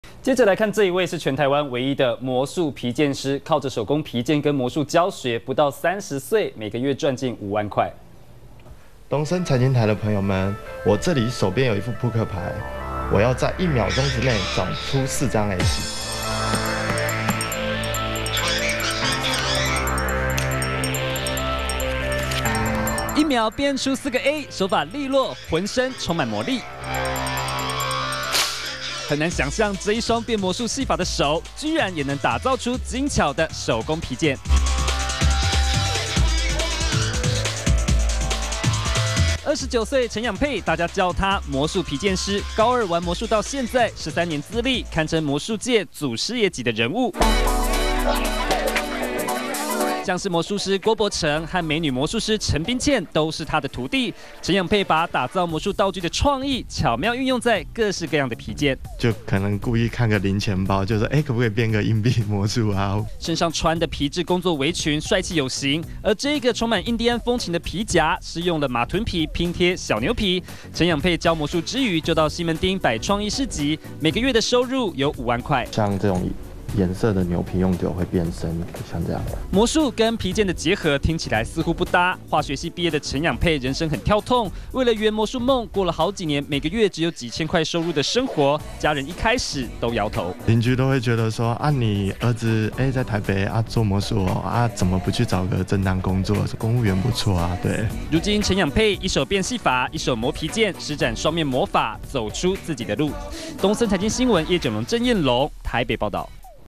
變戲法.磨皮件！七年級魔術皮件師月賺50K
走不一樣的路，也能替自己賺到50K的月收入！帶您來看這一位【魔術皮件師】，29歲的陳仰沛，13年的魔術資歷，堪稱國內魔術界祖師爺等級，三年前他開始把做魔術道具創意，運用在手工皮件，每個禮拜到西門町創意市集擺攤，加上魔術教學，兩種魔力讓他每個月賺進五萬塊！